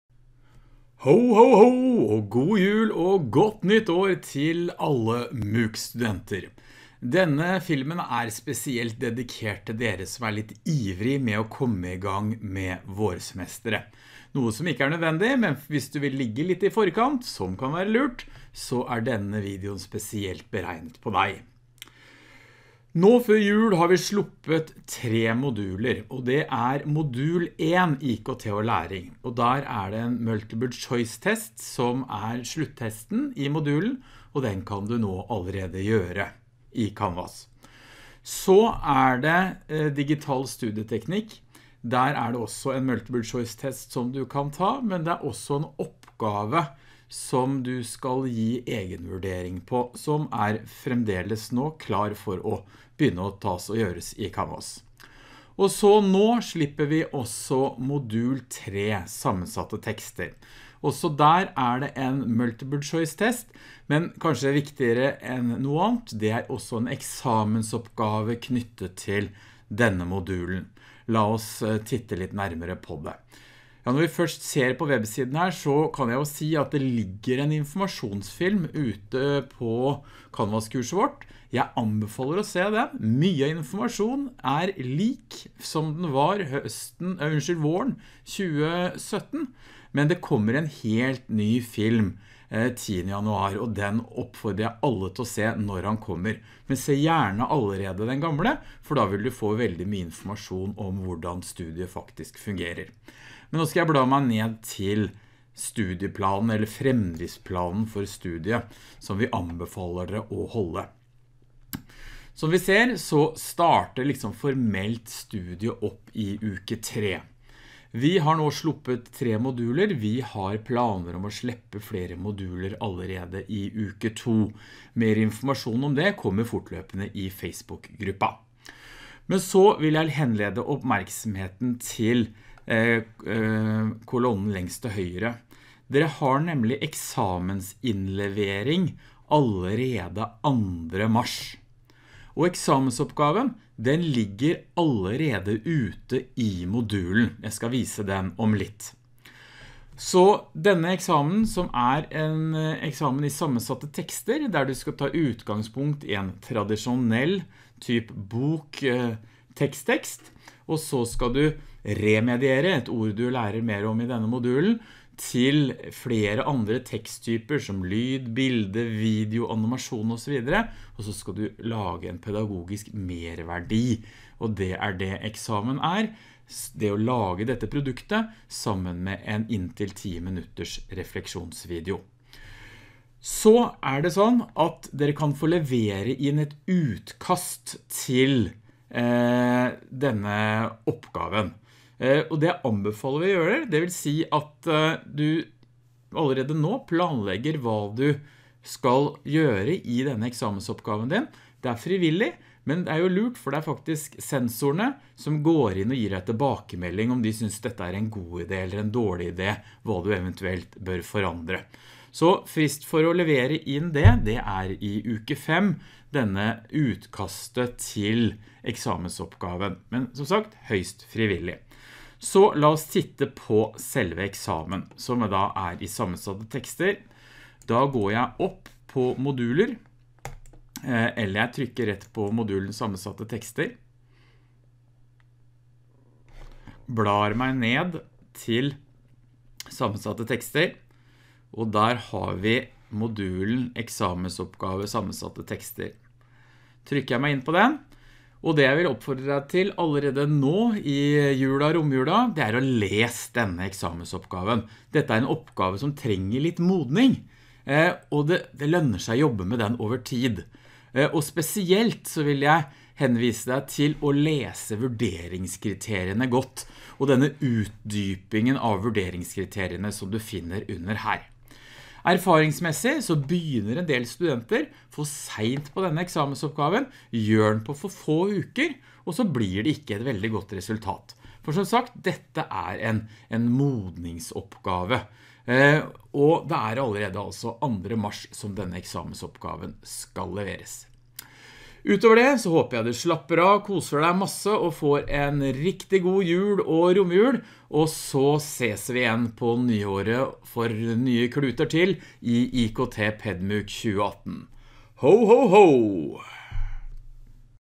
Informasjonsfilm og julehilsen
I denne Informasjonsfilm forteller jeg hva du kan jobbe med frem til studiestart i  januar.